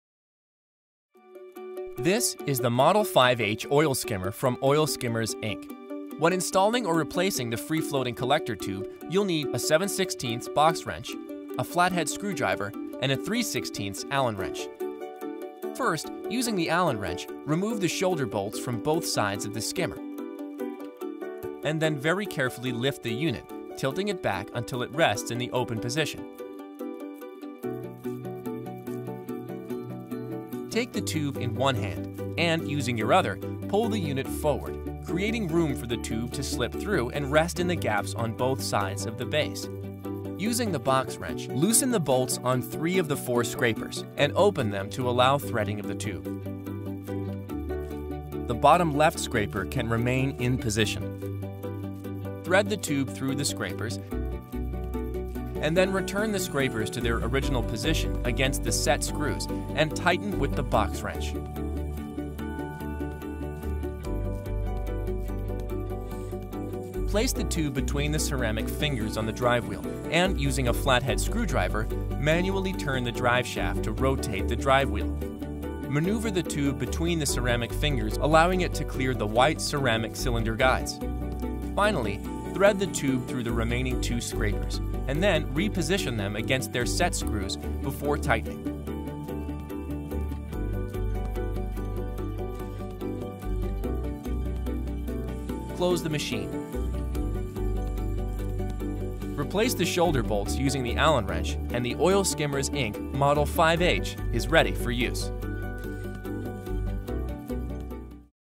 Installing or Replacing the Collector Tube on the Model 5H
See how to replace the free-floating collector tube on the Model 5H oil skimmer from Oil Skimmers, Inc.

The Oil Skimmers, Inc. Model 5H oil skimmer specially designed for small or hard-to-get-at places or in congested plant areas where the unit can be located within 3 feet of the water level. It is fully automatic, works 24/7, and can be installed on open or closed tanks.